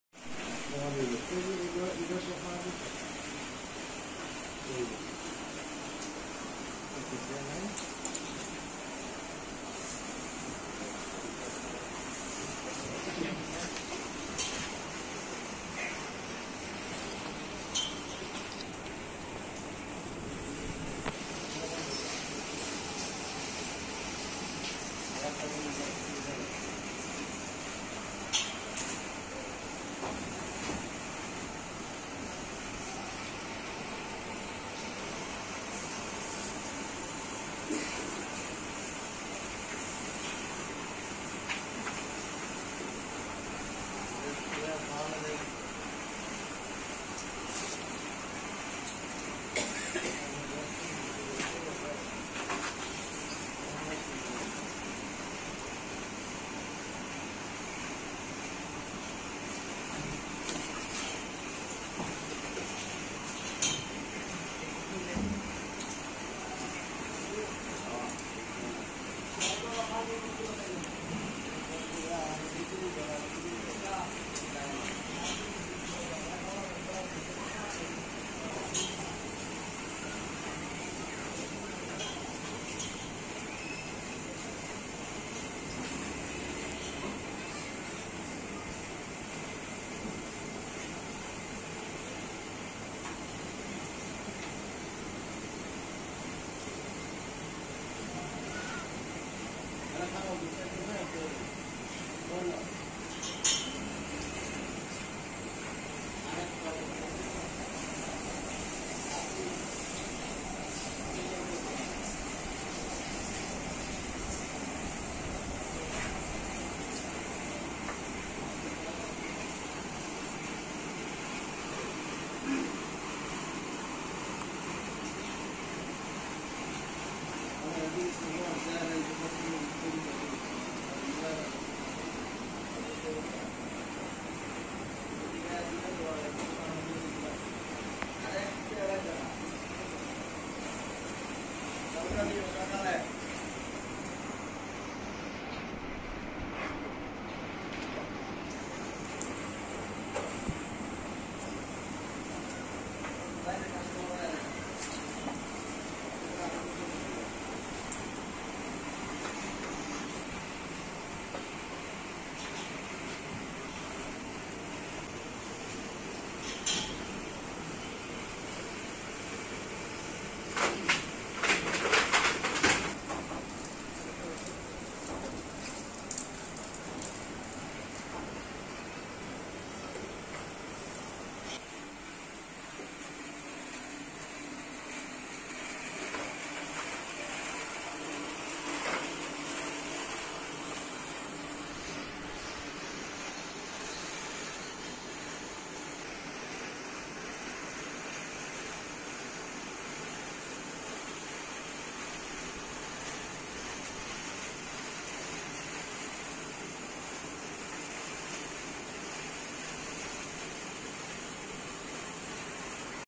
শহরে গরুর খামার | Cow Farm in City | Khilbaritek
শহরে গরুর খামার । ঢাকা শহরে এ রকম অনেক খামার রয়েছে।